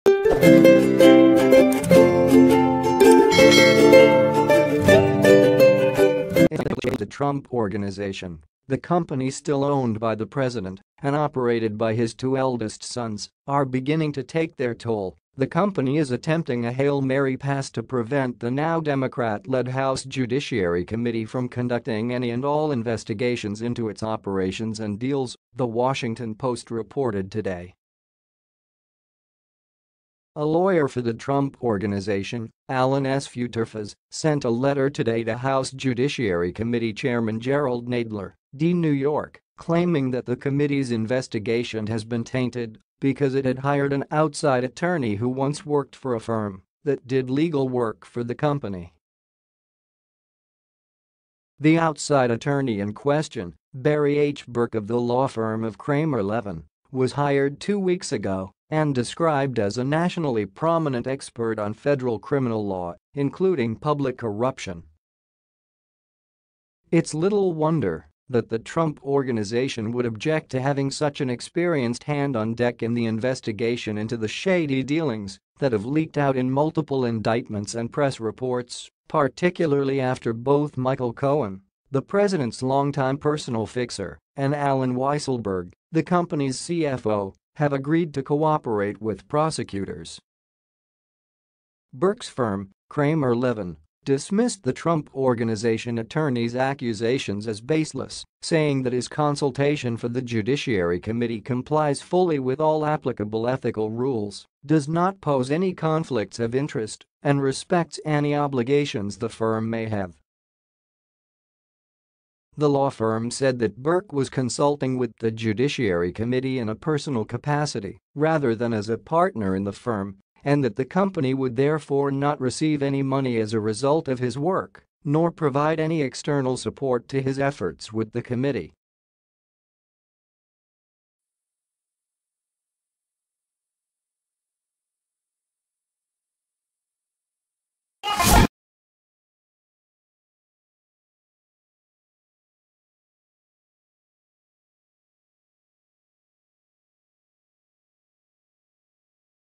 The Trump Organization just ordered House Democrats
In a sign that the pressure of multiple investigations into allegations of decades of illegal activities by the Trump Organization — the company still owned by the president and operated by his two el